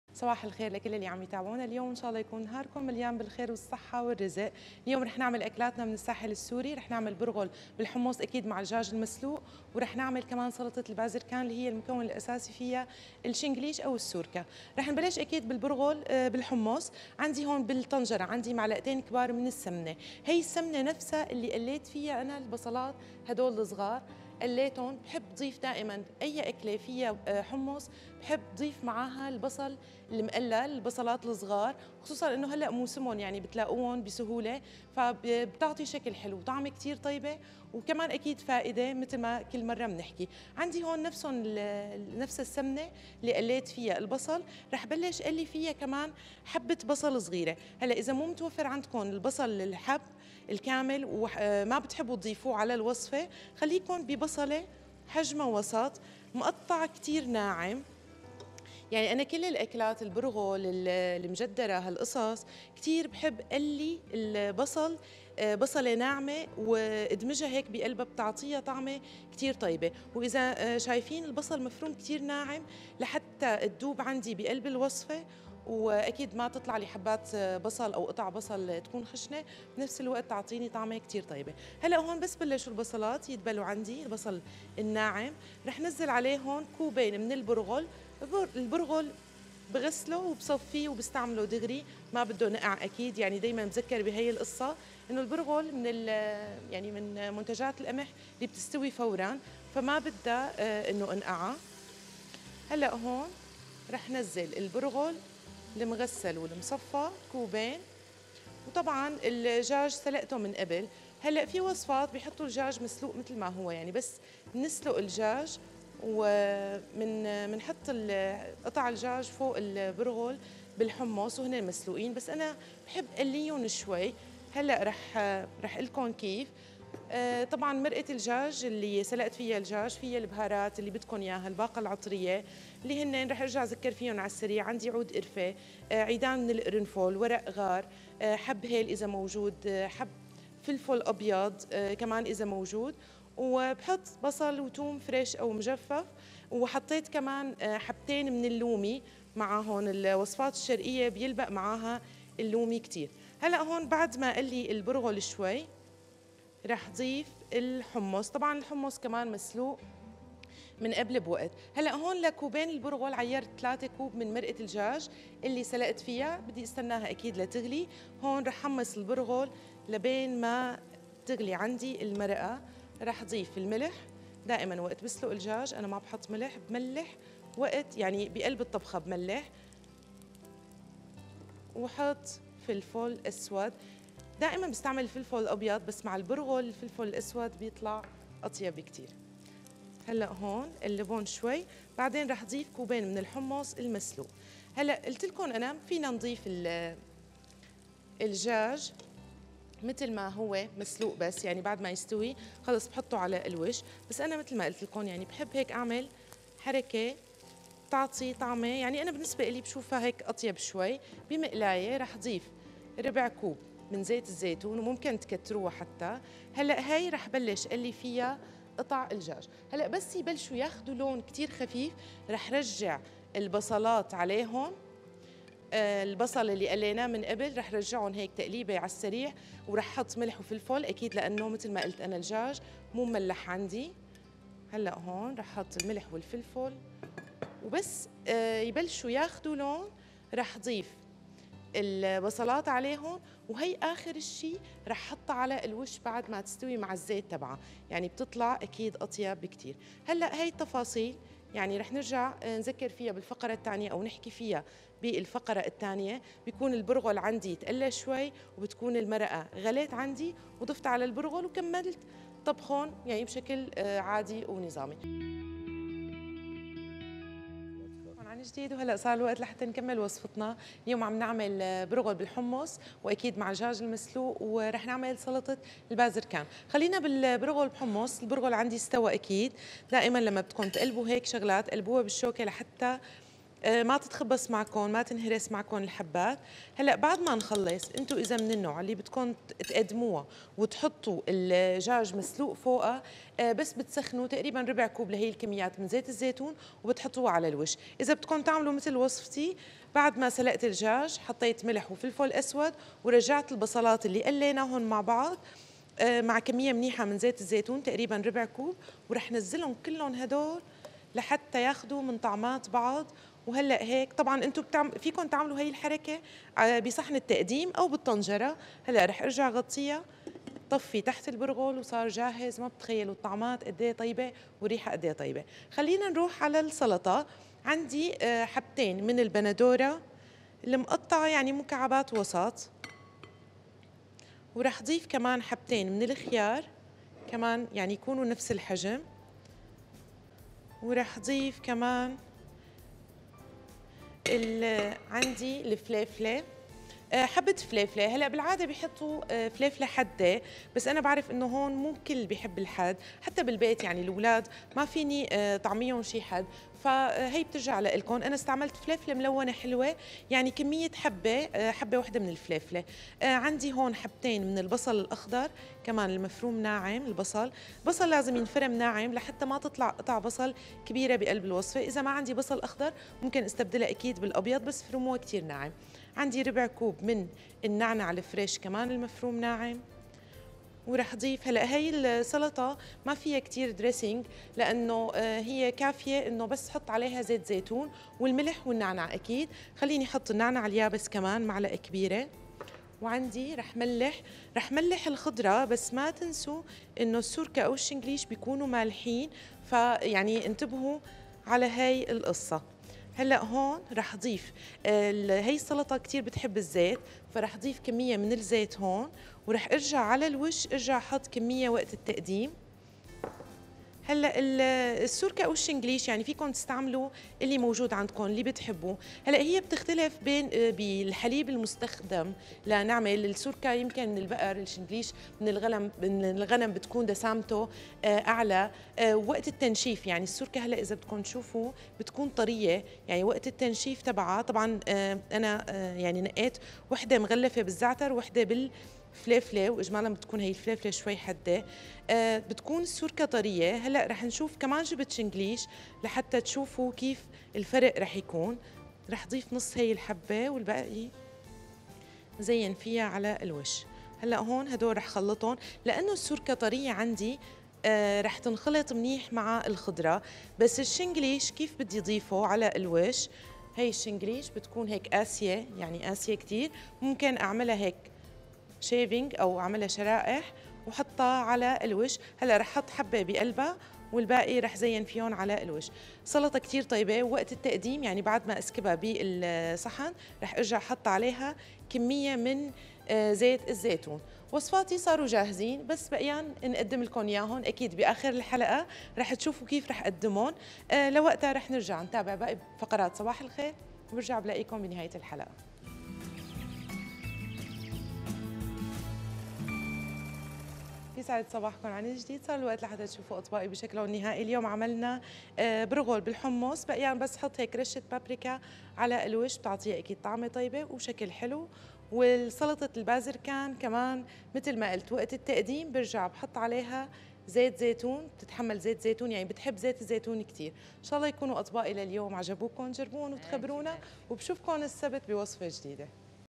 طبخة اليوم "برغل بالحمص + سلطة بازركان "
مقادير البرغل بالحمص  :
نصف كيلو دجاج
باقة عطرية
2 ملعقة كبيرة سمنة
نصف  كيلو بصل حجم صغير
ربع  كوب زيت زيتون
  بصلة
2 كوب برغل خشن
2 كوب حمص مسلوق
3 كوب مرقة دجاج

سلطة البازركان:
2 حبة ( بندورة، خيار ، بصل أخضر)
  حبة فليفلة
ربع كوب نعناع
2 حبة سوركة
ربع  كوب زيت زيتون  ملعقة صغيرة نعناع يابس
ملح